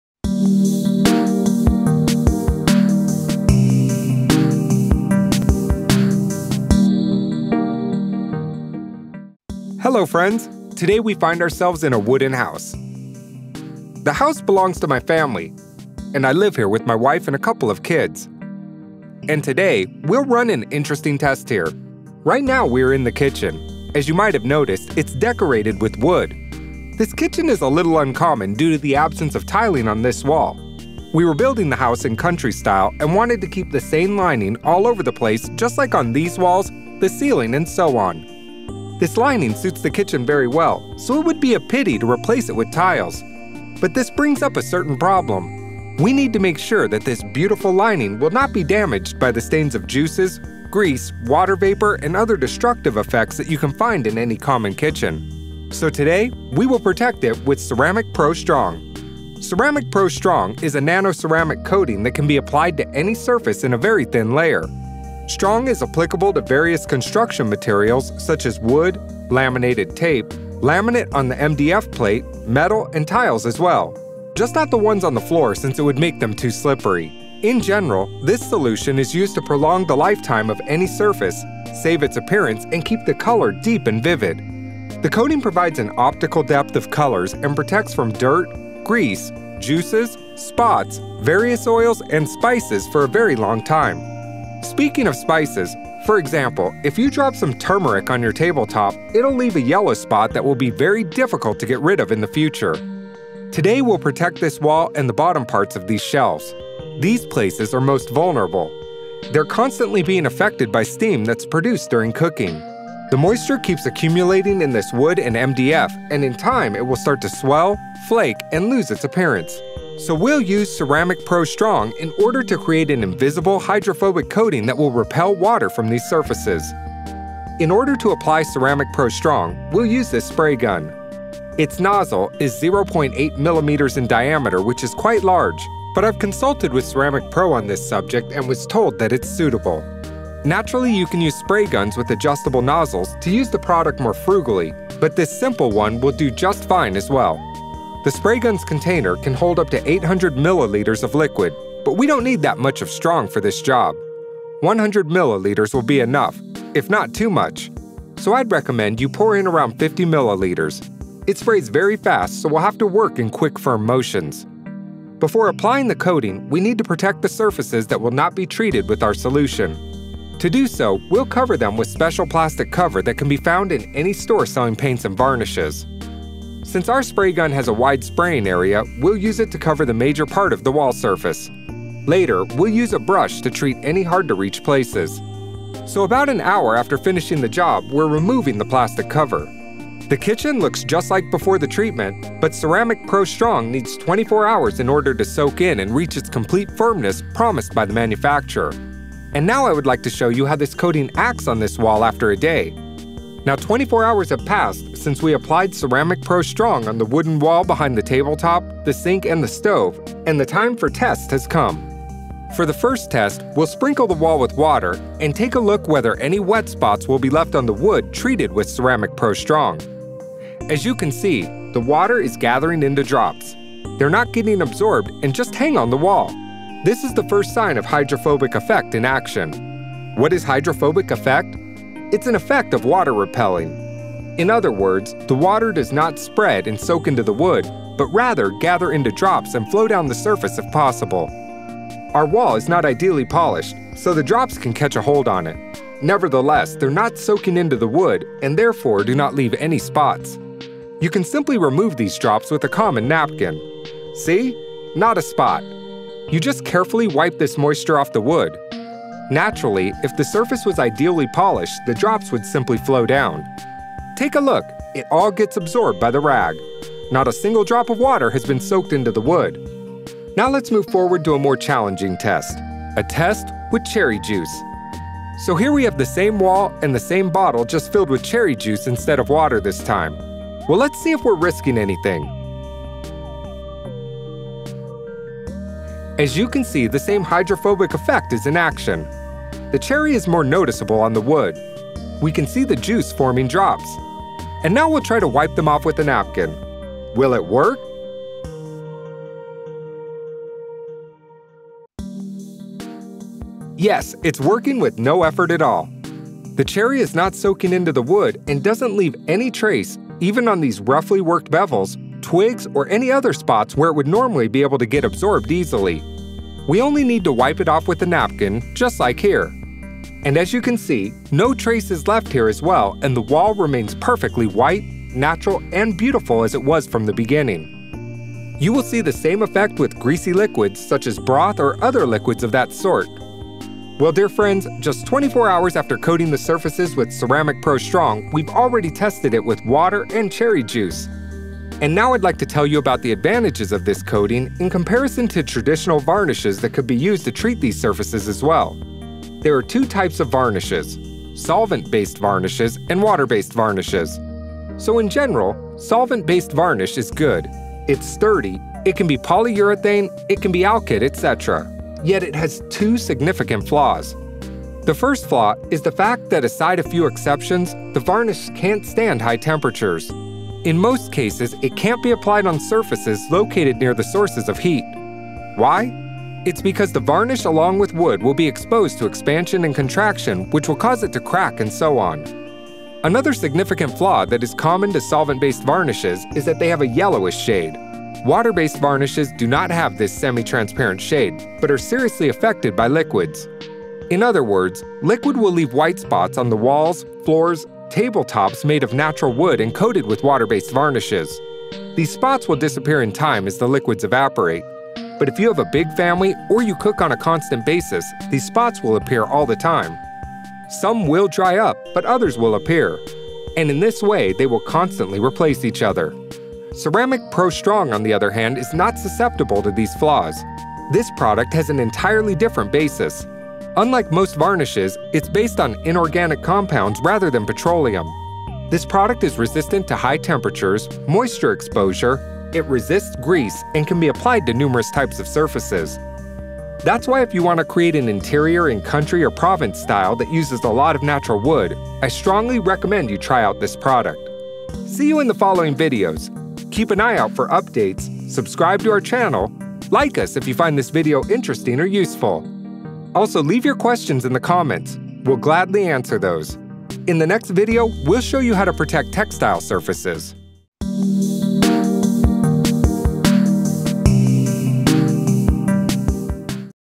Ceramic Pro Strong for Wooden Surfaces
How to protect wooden interior from spots with Ceramic Pro Strong